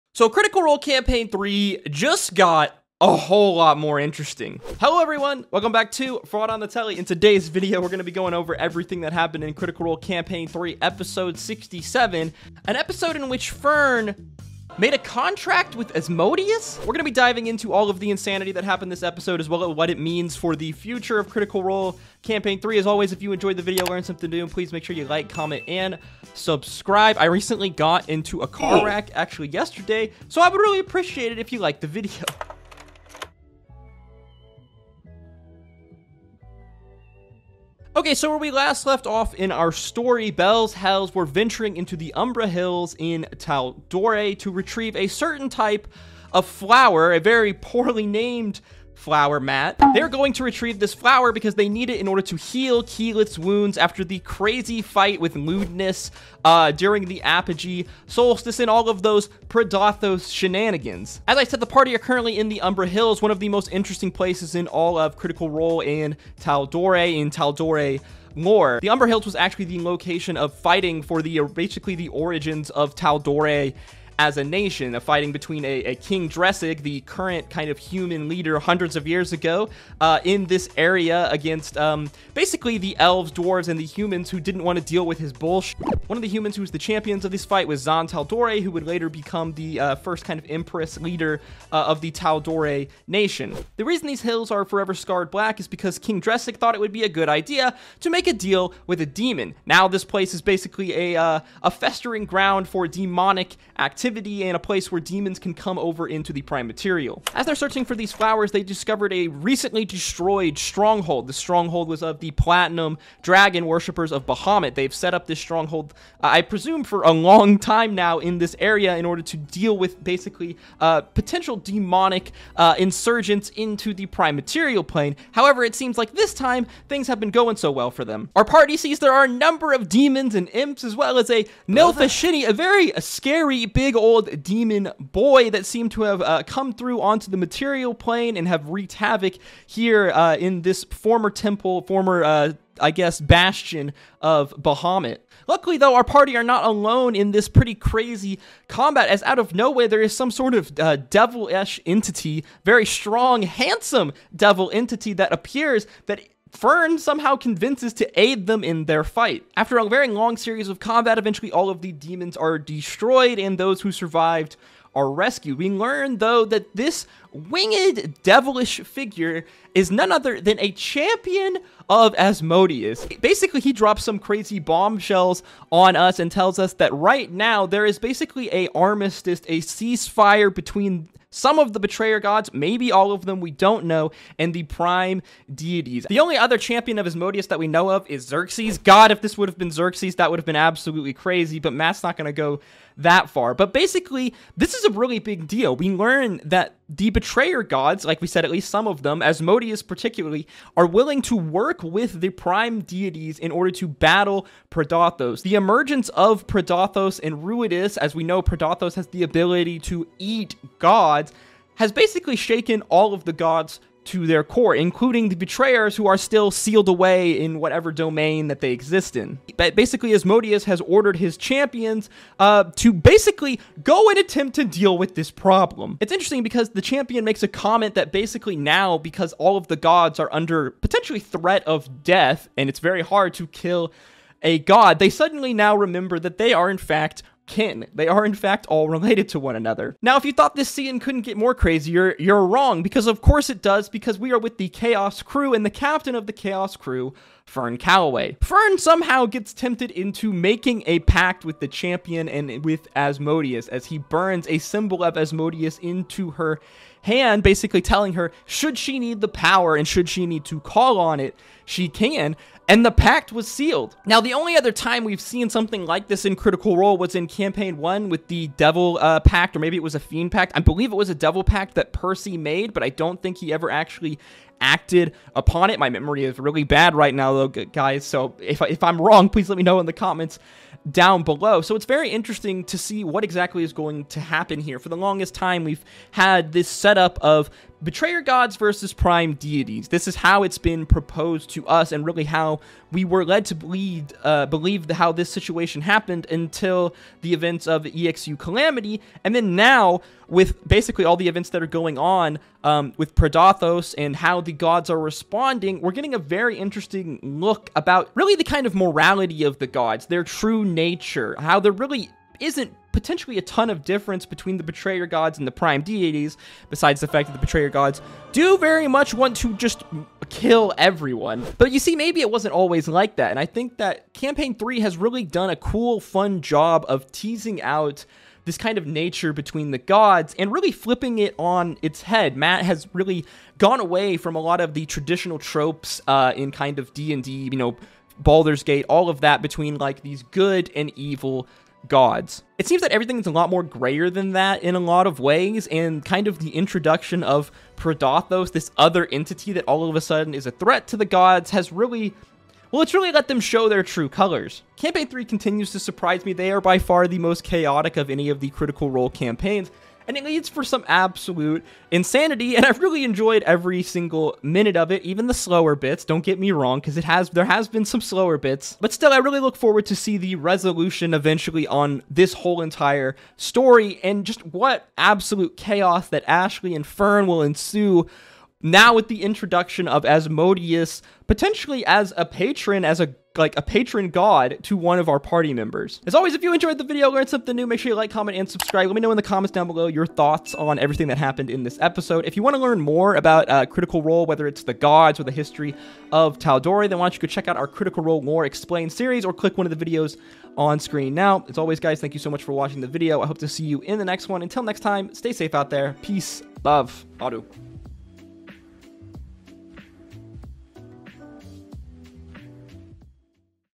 Critical Role Campaign 3 Just Got REALLY Interesting
Critical Role Campaign 3 Just Got REALLY Interesting...in this video we discuss Critical Role Campaign 3 episode 67, an episode with an unlikely deal with Asmodeus.

FraudOnTheTele is a media commentary and entertainment channel, where we breakdown shows, anime, dnd, and trending topics.